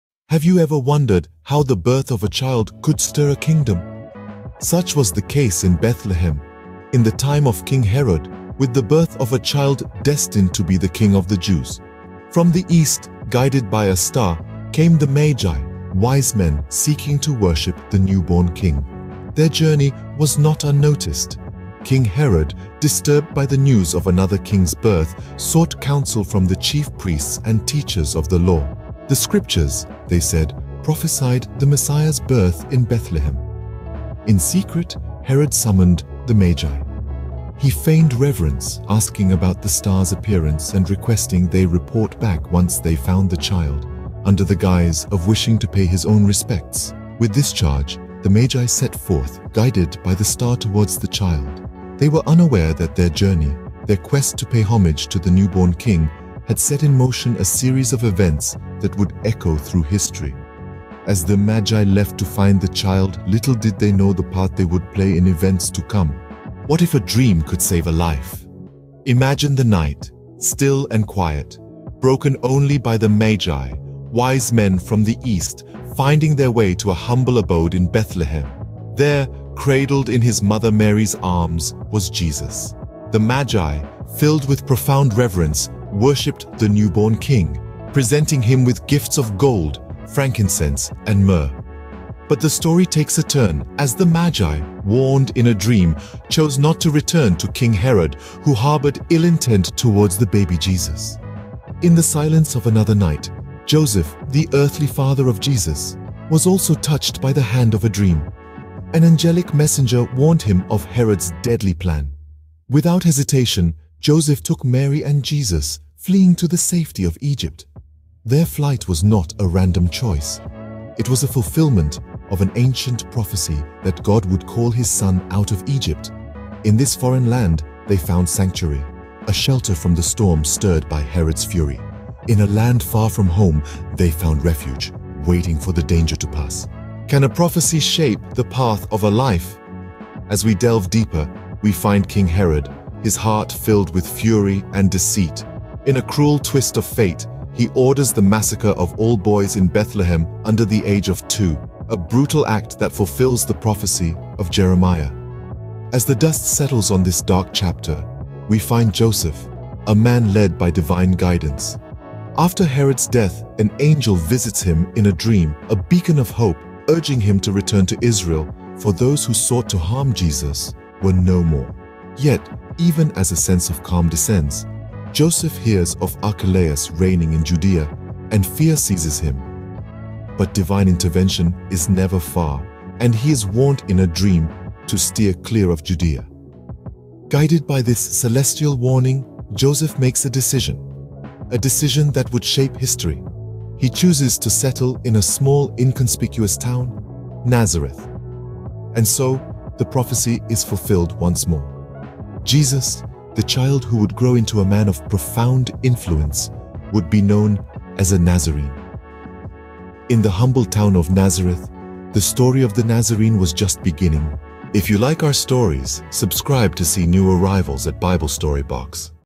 Matthew Two: Journey of the Wise Men
Join us on an intriguing and enlightening journey as we unravel the untold story of the wise men in Matthew Two. Discover the secrets behind their captivating journey, starting from their homeland, following a celestial phenomenon and eventually leading to the birth of Jesus. This never-before-seen perspective on the biblical story of the wise men will leave you astounded and inspired. Brace yourself for surprises, revelations, and historical insights that will forever change your perception of this significant biblical occurrence. Don't miss this opportunity to explore the fascinating tale of the wise men's trek and the profound impact they made on the world. Watch now and be enthralled!